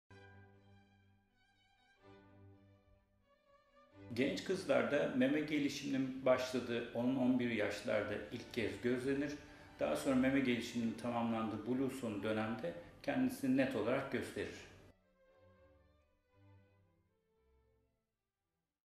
Tübüler meme ne zaman oluşur? - Prof. Dr. Tayfun Aköz
Prof. Dr. Tayfun Aköz “Tübüler meme ne zaman oluşur?” sorusunun yanıtını veriyor.